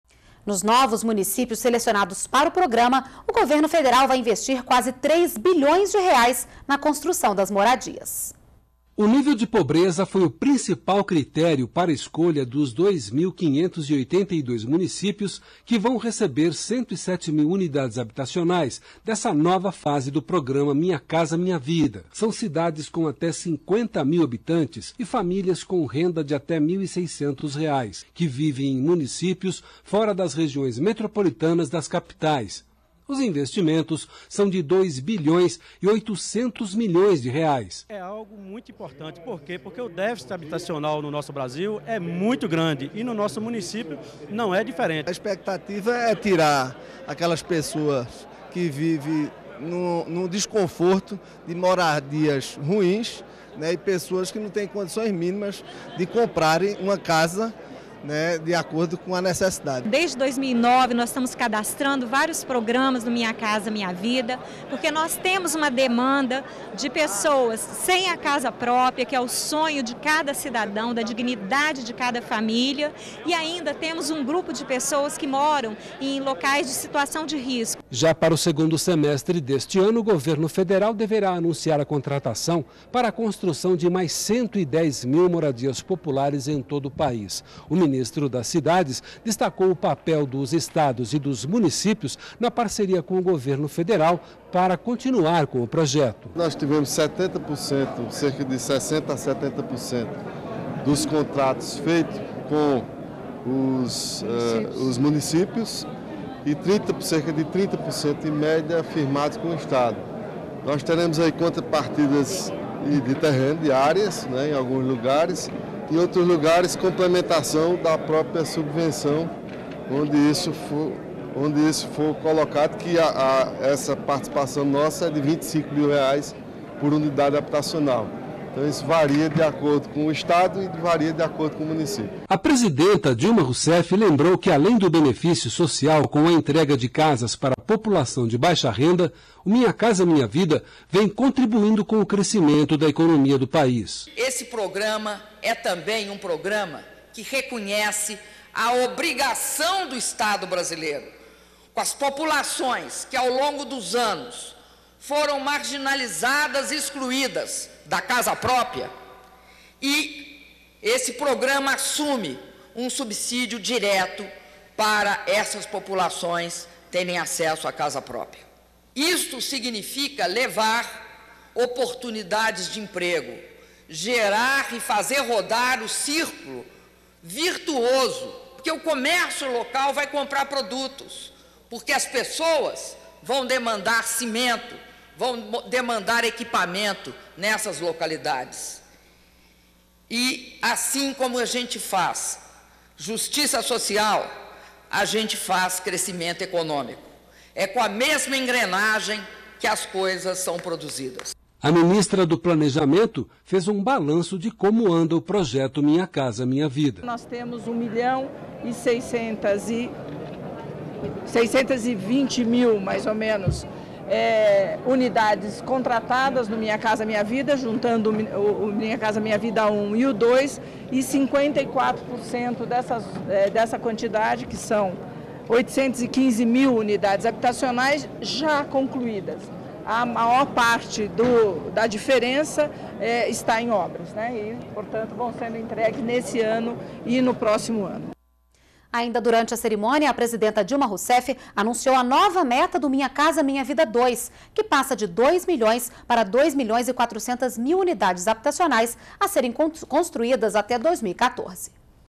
Governo vai investir R$ 3 bilhões na construção de moradias do Minha Casa, Minha Vida 2
NBR NOTÍCIAS - 12.04.12: Nos novos municípios selecionados para o programa, o governo federal vai investir quase R$ 3 bilhões na construção das moradias na nova fase do programa Minha Casa, Minha Vida. Ainda durante a cerimônia, a presidenta Dilma Rousseff anunciou a nova meta do Minha Casa, Minha Vida 2, que passa de 2 milhões para 2,4 milhões unidades habitacionais, a serem construídas até 2014.